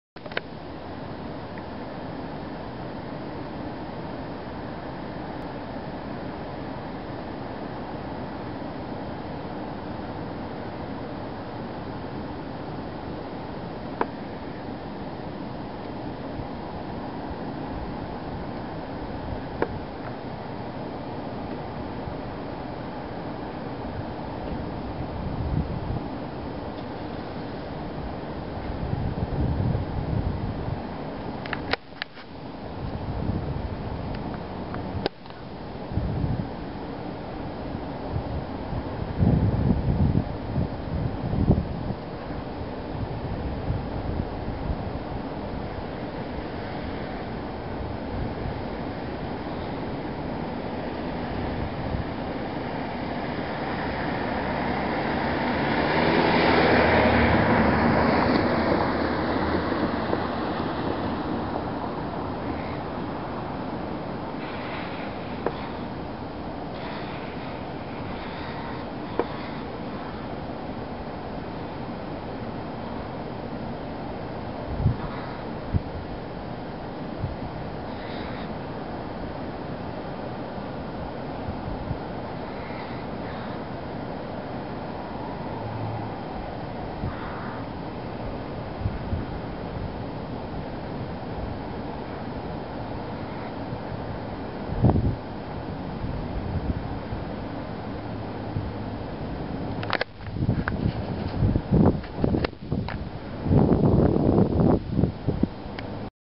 Corneilles vs urubus - École Polytechnique de Montréal (vidéo 2)
Vidéo filmée à 18h40, le 12 avril 2014.
Contexte: Au moins 17 corneilles attaquaient 4 urubus.
On voit en particulier dans cette vidéo un urubu avec 6 corneilles à ses trousses.